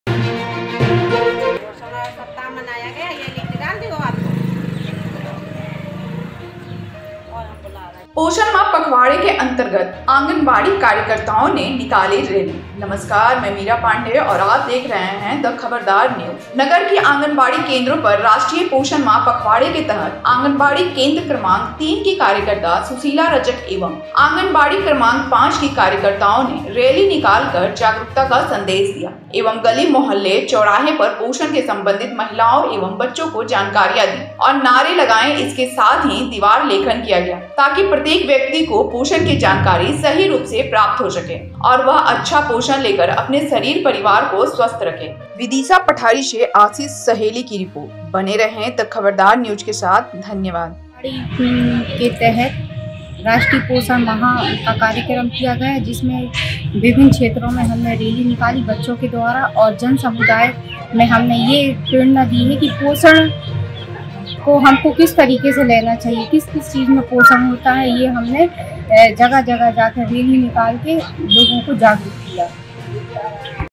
पोषण माह पखवाड़े के अंतर्गत आंगनबाड़ी कार्यकर्ताओं ने निकाली रैली
विदिशा के पठारी । नगर की आंगनवाड़ी केंद्रों पर राष्ट्रीय पोषण माह पखवाड़े के तहत आंगनवाड़ी केंद्र क्रमांक 3 कि कार्यकर्ता सुशीला रजक एवं आंगनवाड़ी क्रमांक 5 की कार्यकर्ताओं ने रैली निकालकर जागरूकता का संदेश दिया एवं गली मोहल्ले चौराहों पर पोषण के संबंधित महिलाओं एवं बच्चों को जानकारियां दी और नारे लगाए इसके साथ ही दीवार लेखन किया गया ताकि प्रत्येक व्यक्ति को पोषण की जानकारी सही रूप से प्राप्त हो सके और वह अच्छा पोषण लेकर अपने शरीर परिवार को स्वस्थ रखसके आंगनबाड़ी कार्यकर्ता सुशीला रजक पठारी 3 एवं रेखा शुक्ला पठारी 5